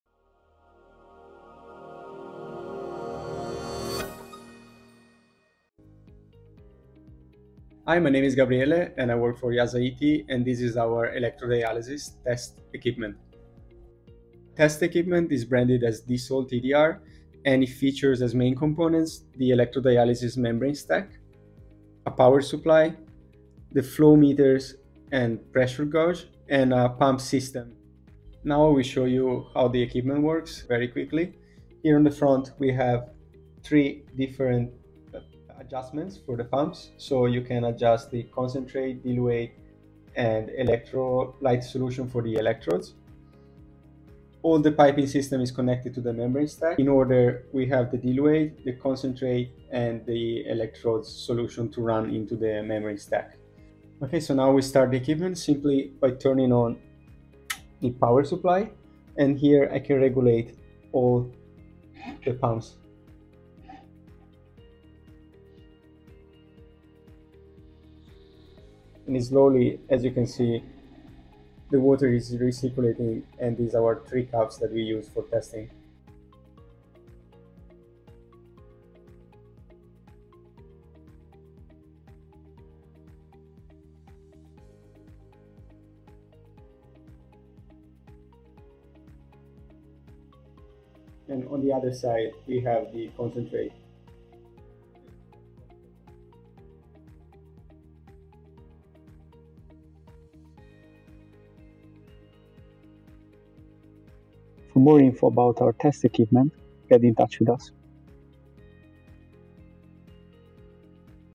Discover DESALT ED/EDR/RED Test Equipment Bench Test Unit Pilot Plant for Electrodialysis Reversal
💡In this video, we dive deep into DESALT ED/EDR/RED Test Equipment Bench Test Unit for Electrodialysis Reversal.

📖 We are providing you with an introduction to understanding and utilizing our Electrodialysis Reversal testing technology.

🔎 We'll explore its features, benefits, and applications in various industries.

🔔 Timestamps:
00:00 - Introduction
00:17 - Test Equipment Features
00:53 - Test Beginning
01:26 - Results